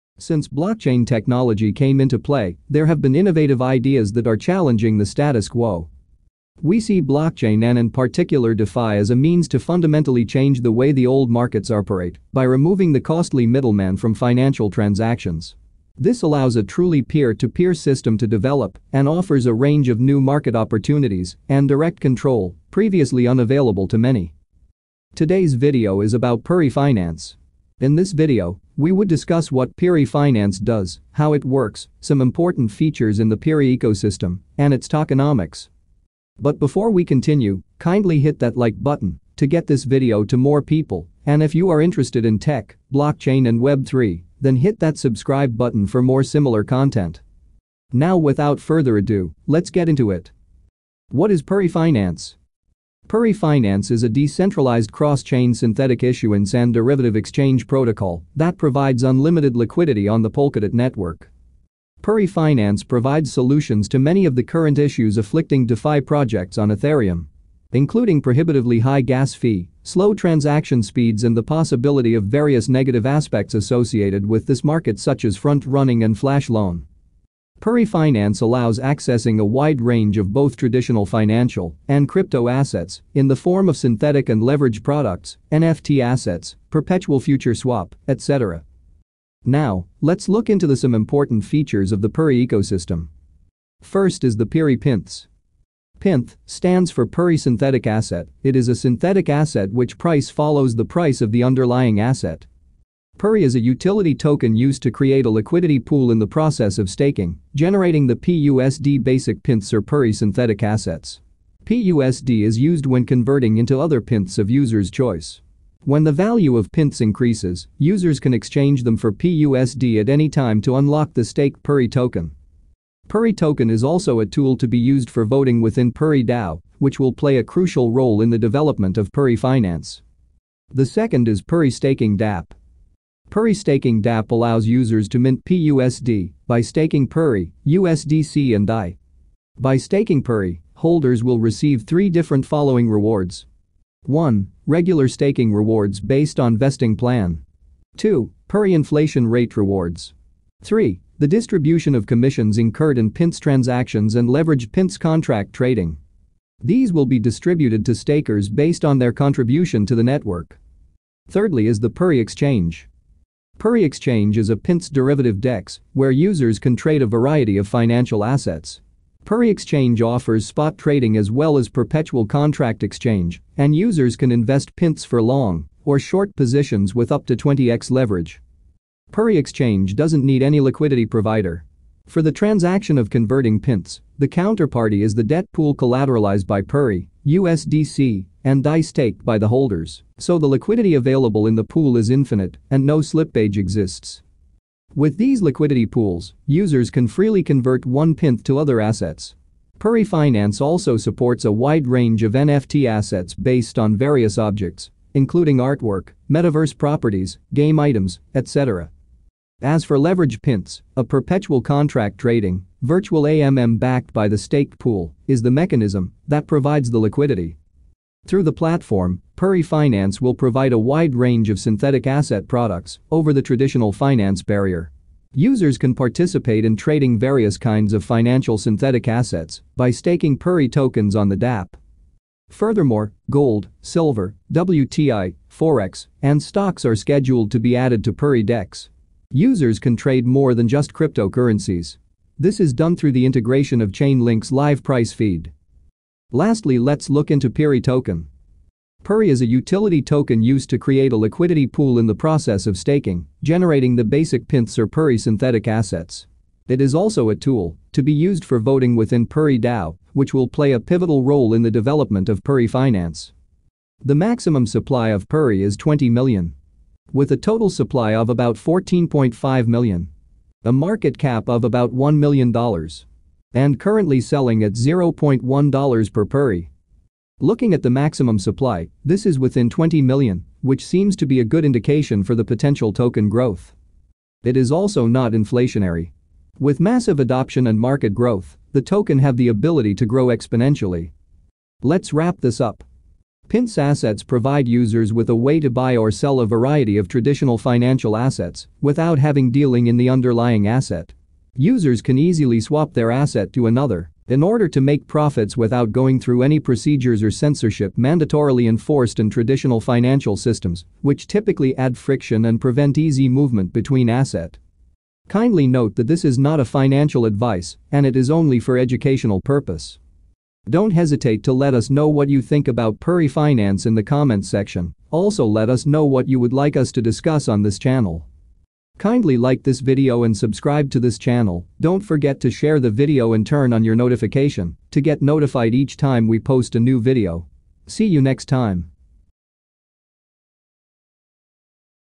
What is Peri Finance? Peri Pynth Explained with Animation
Everything you need to know about PERI Finance, the Peri Ecosystemand Tokenimoics.
Don't hesitate to share your ideas in the comment section...

0:53 what is Peri Finance
1:37 what is Peri Pynth
2:13 Peri Staking dApp
2:42 Peri Exchange
4:09 Peri Tokenomics